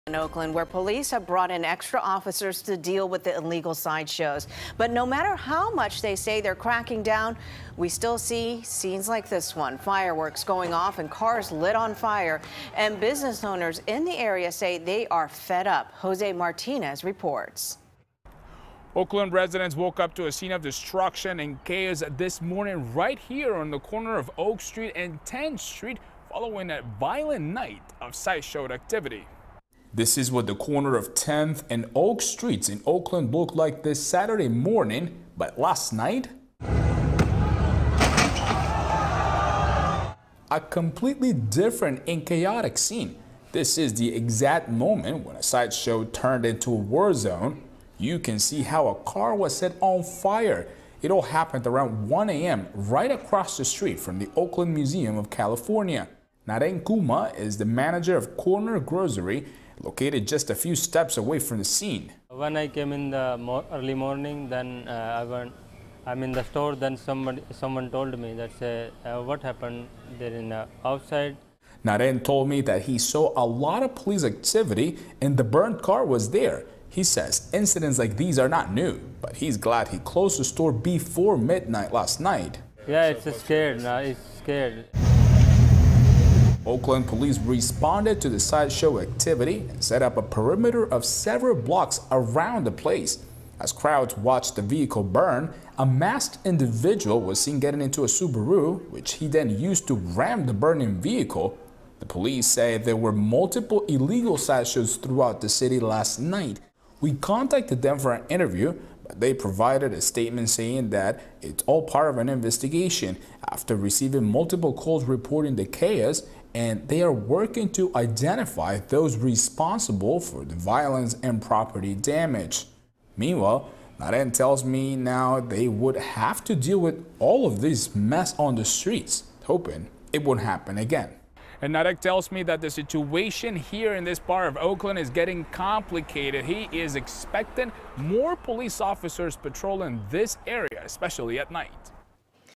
Fiery Oakland sideshows leave city streets resembling a war zone
Oakland residents woke up to a scene of destruction Saturday morning at  the corner of Oak Street and Tenth following a violent night of sideshow activity. José Martínez reports. (5-6-23)